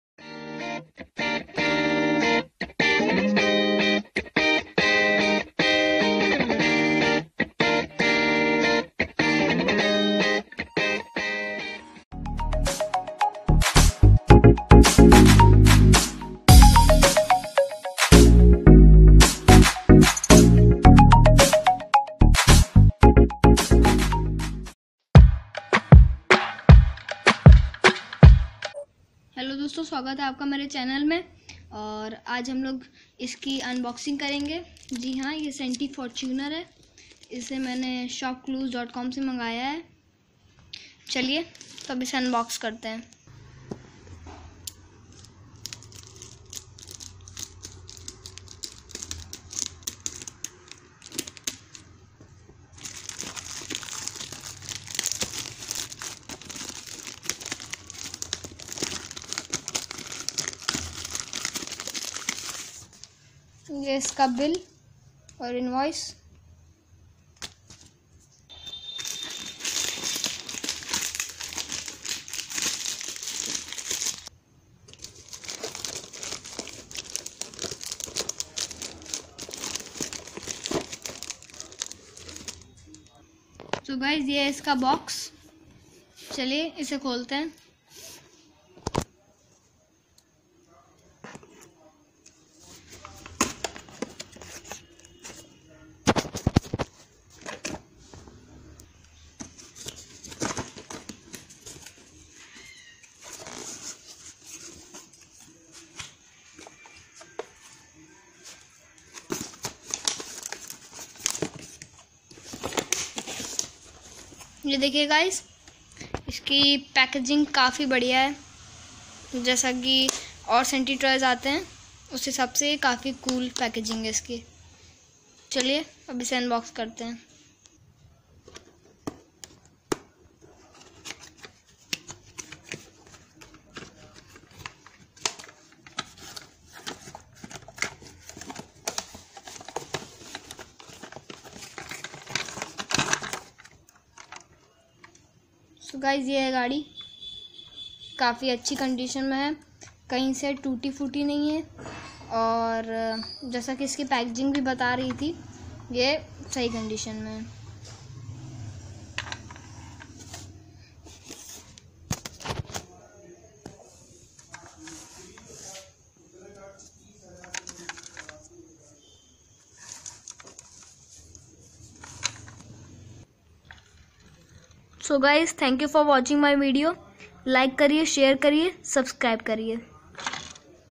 Unboxing My New Centy Toys Toyota Fortuner| Sky Blue Colour| Model by Toyota Merchandise🔥I Me Myself
Ordered from SHOPCLUES.COM